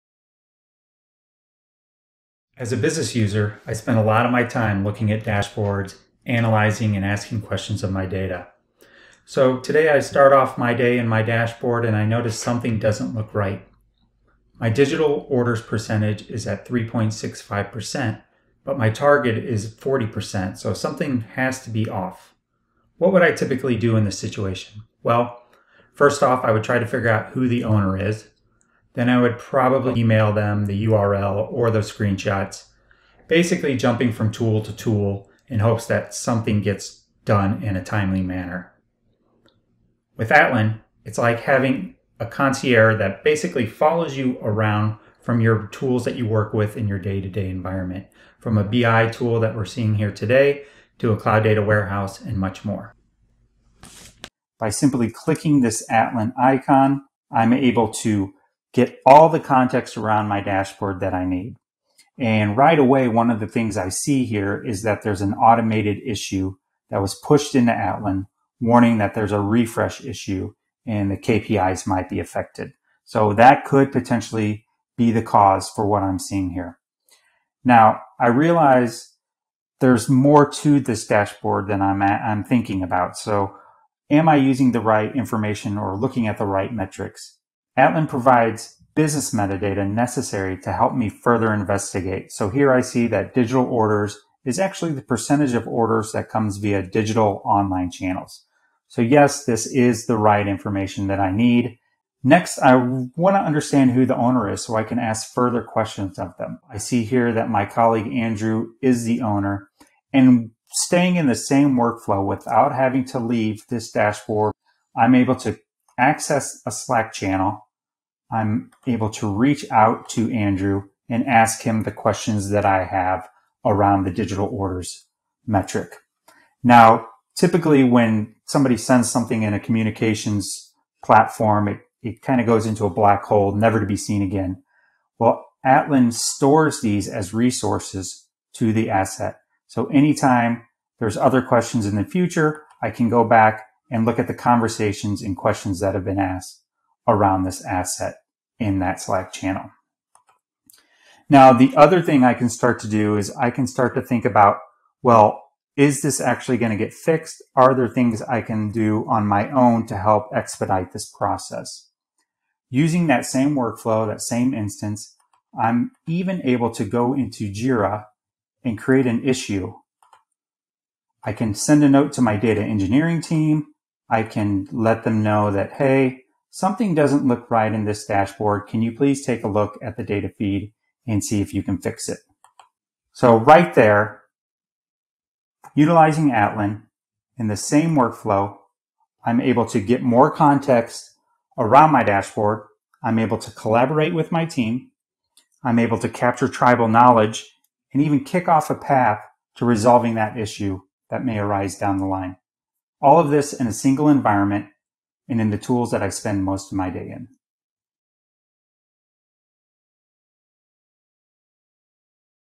Atlan Demo Series — The Atlan Chrome Extension
Avoid application overload, and stay in your flow. Atlan brings the data and context right where you spend most of your time, in your everyday applications.